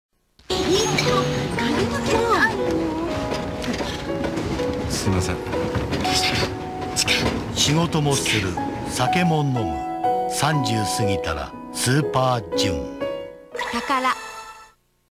宝酒造 スーパー純 舘ひろし 懐かCM 1993年7月 TaKaRa
満員電車で痴漢に間違われる舘ひろしが切ないな～。